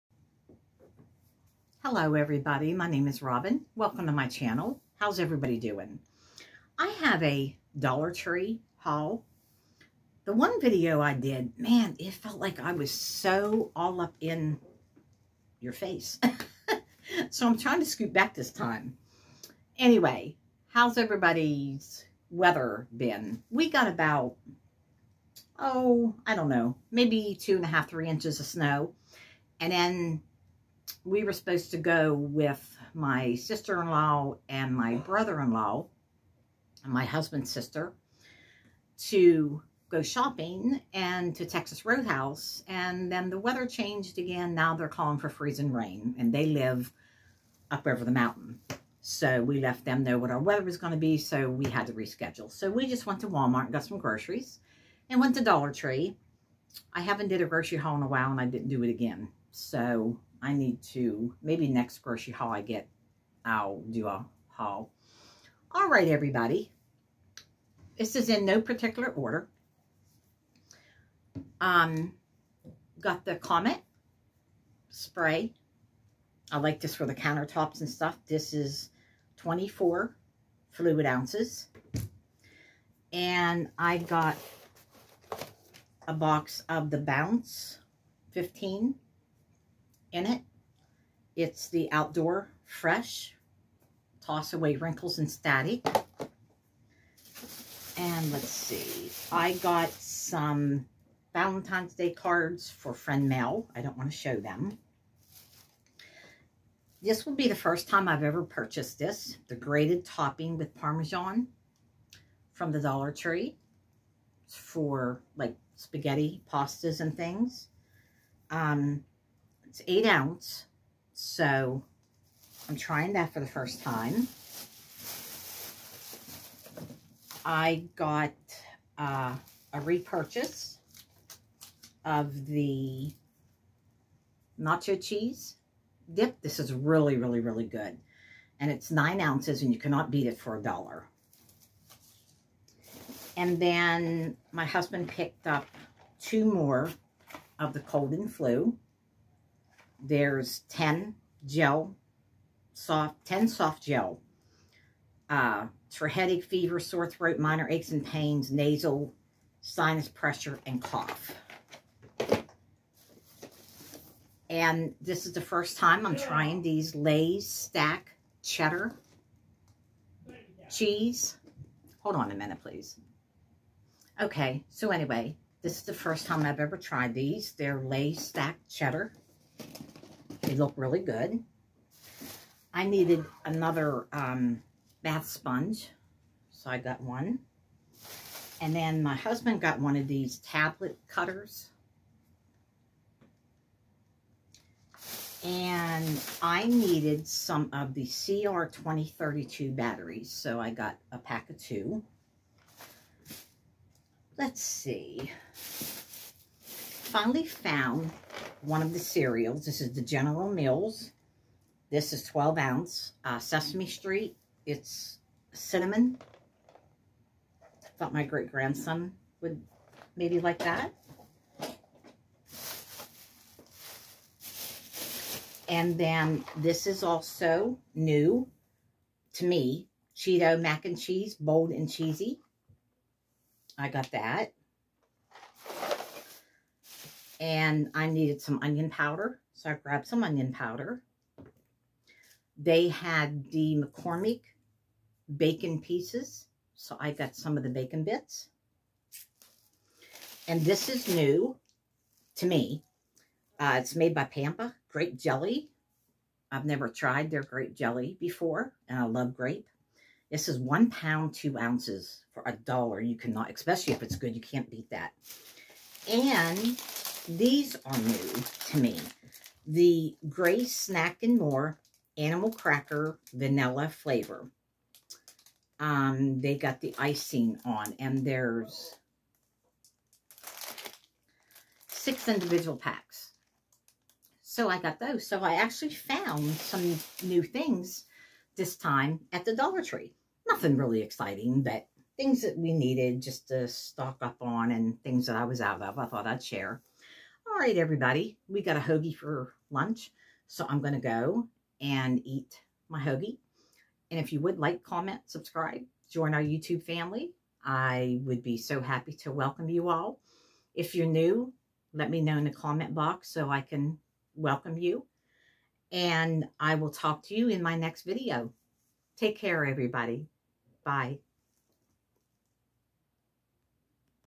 Dollar tree haul with new products I found.
As always thank you for taking time out of your day to stop by my Channel, watch, and leave a comment I so greatly appreciate each and everyone of you. I found some new things at my Dollar Tree.

Address
Robin Sadowsky